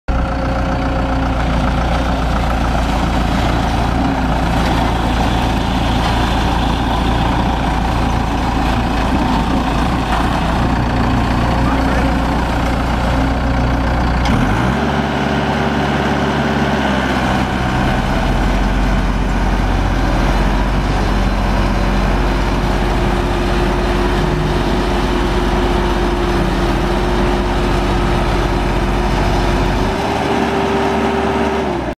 1996 Hypac C778A double drum vibratory roller for sale at auction | bidding closes August 8, 2019
1996 Hypac C778A double drum vibratory roller
2,518 hours on meter
Cummins 4B3.9 four cylinder diesel engine
130 HP
Hydrostatic transmission
OROPS
Vandal cover
78"W x 48" diameter smooth drums
Dual two speed vibratory system
Independent drum controls
Dual propel system
Serial A215C1585T
Unit# 7203
It's located in St. Louis, MO, and we're selling it to the highest bidder regardless of price.